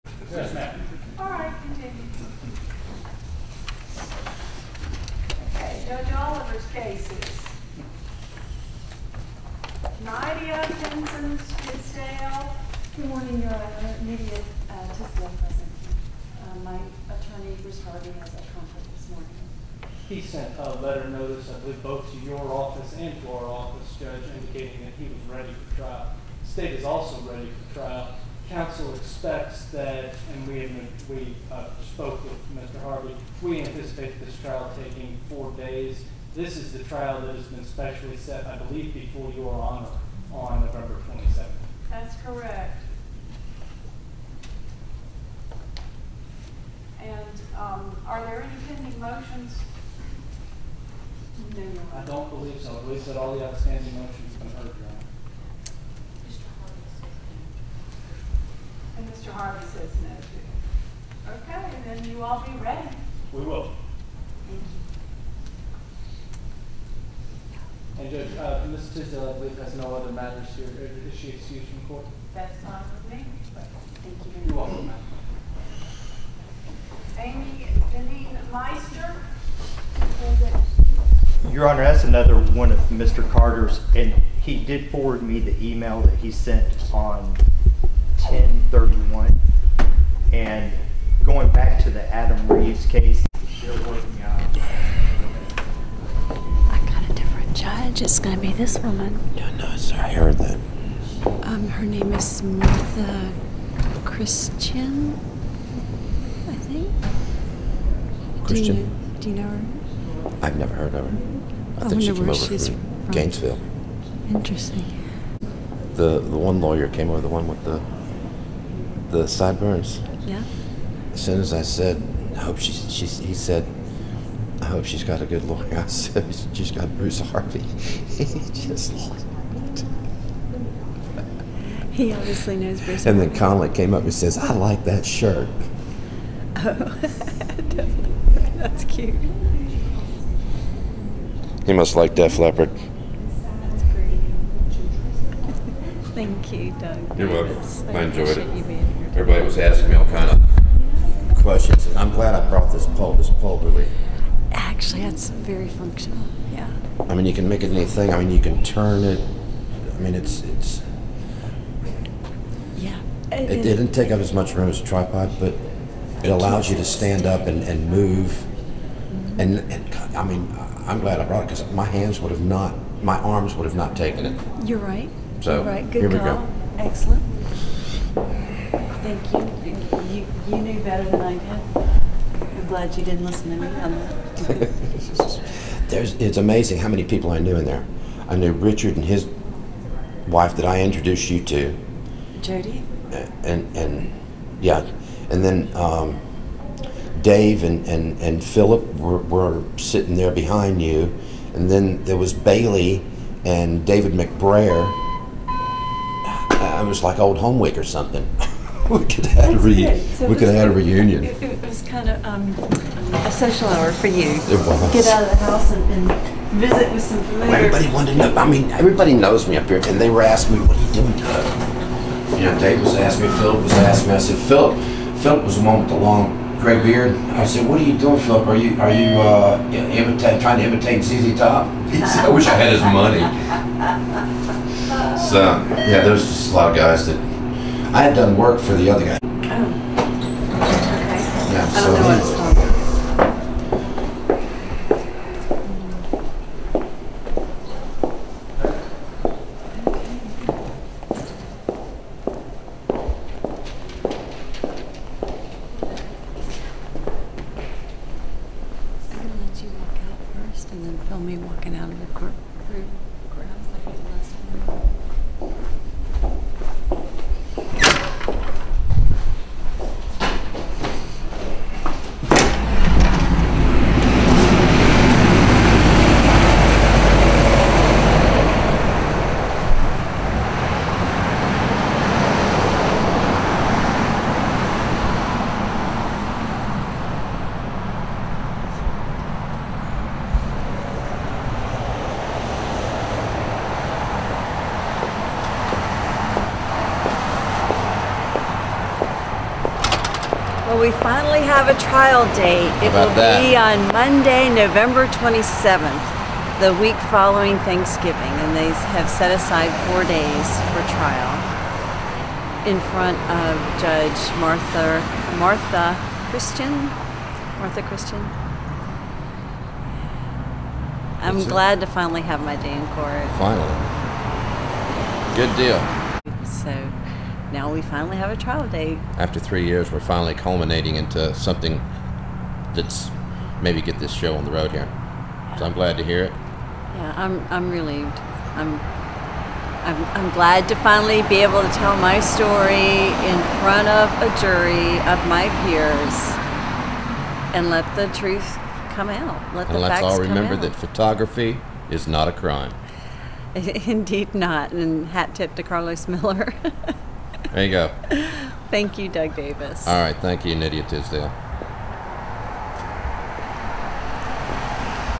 Nydia Tisdale trial specially set for Nov. 27–30 before Hon. Sr. Judge Martha Christian 11-02-17
Nydia Tisdale trial specially set for Nov. 27–30, 2017 before Honorable Senior Judge Martha Christian in Dawson County Superior Court, 25 Justice Way, Dawsonville, Georgia 30534.

The trial is open to the public to attend.